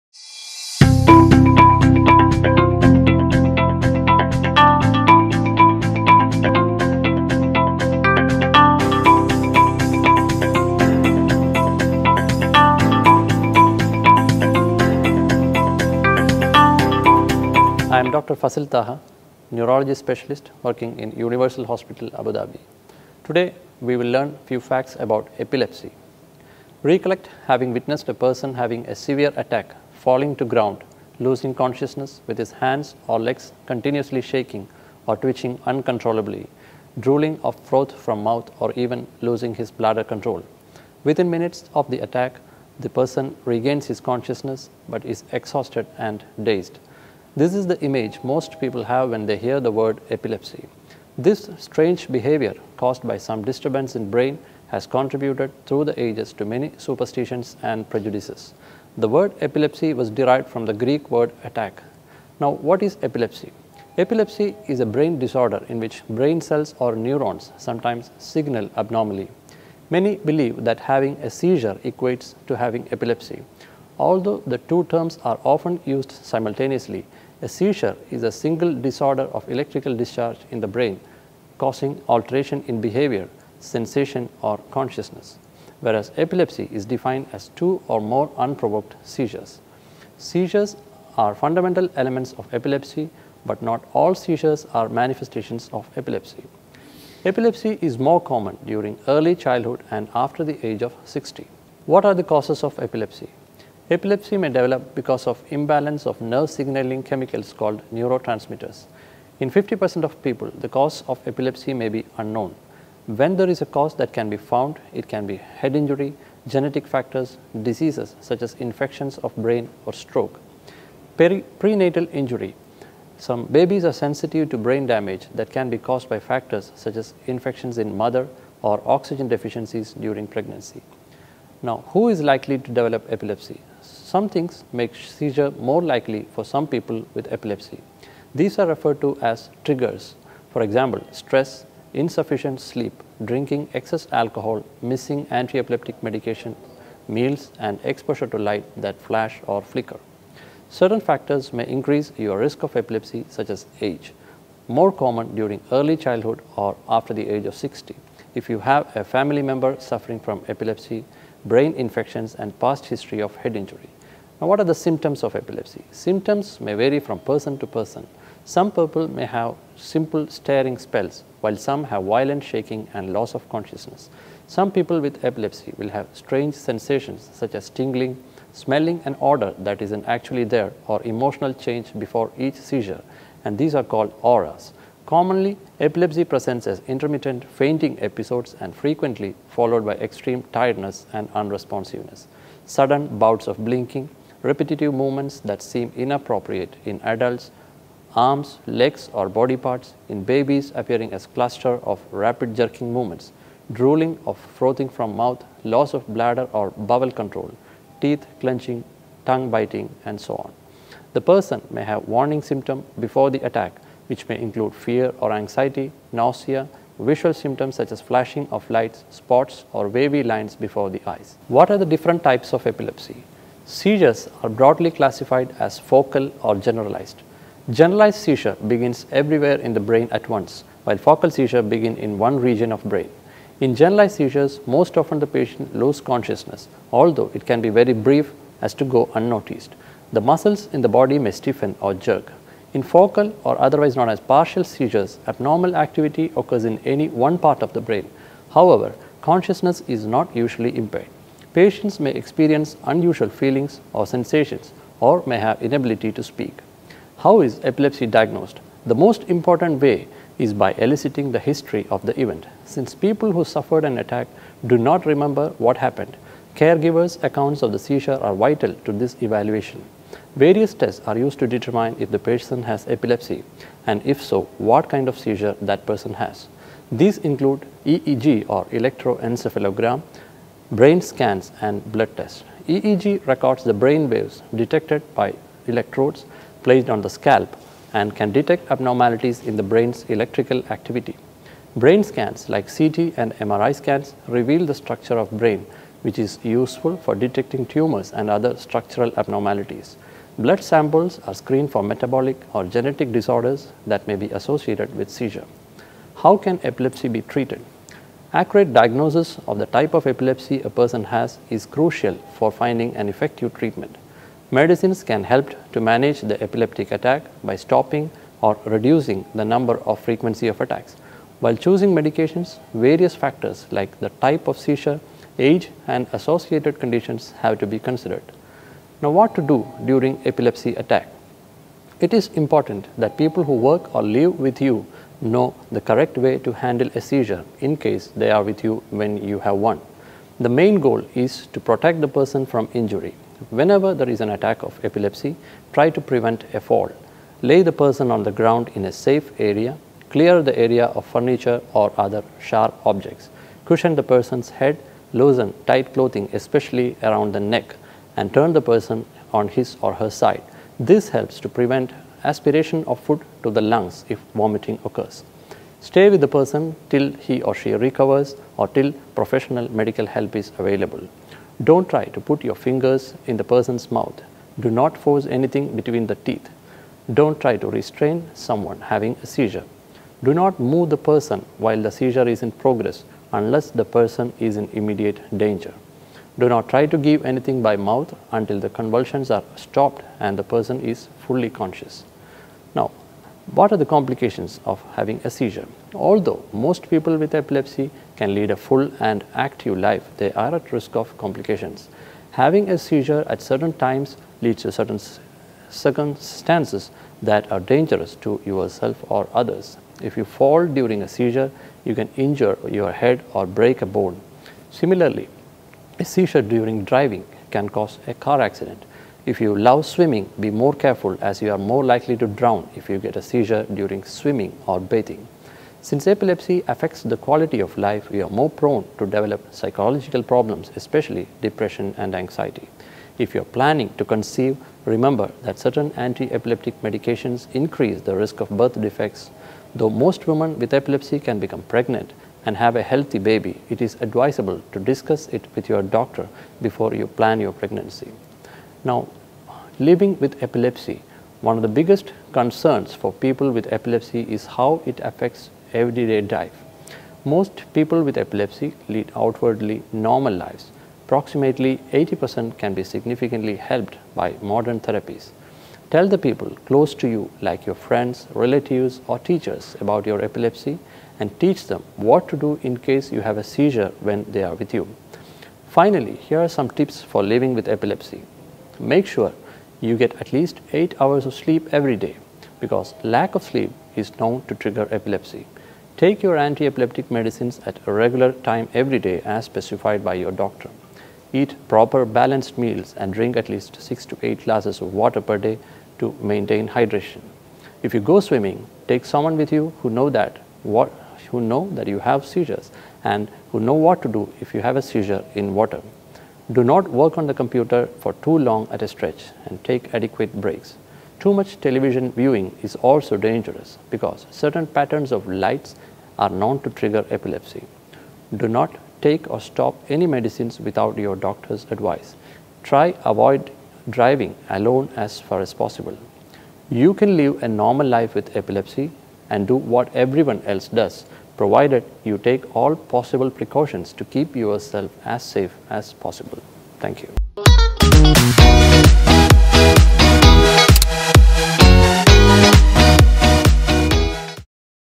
Heart to Heart with Dr Fazil Thaha - Specialist Neurology
Epilepsy, the fourth most common neurological disorder affecting people of all ages, is caused by a disturbance in the brain where the neurons signal abnormally. Dr Fazil Thaha, Neurologist at Universal Hospital, talks about Epilepsy and clears out its difference with a seizure.

Epilepsy is characterized by unpredictable, unprovoked, and recurrent seizures and it is more common in early childhood or after the age of 60. This can be caused by head injuries, genetic factors, underlying diseases, prenatal injury, etc. Triggers such as stress, insufficient sleep, excessive alcohol intake, missing drug doses, exposure to light and the like can also induce a seizure. Symptoms vary from every person. Some may display certain “auras” such as staring spells, shaking, or loss of consciousness followed by the fainting episodes, rapid jerking of body parts, drooling, or teeth clenching. After the attack, the person regains consciousness but will be exhausted.

Dr. Fazil Thaha also discusses the types of seizures, diagnosis and complications, treatment and how to safely handle a seizure.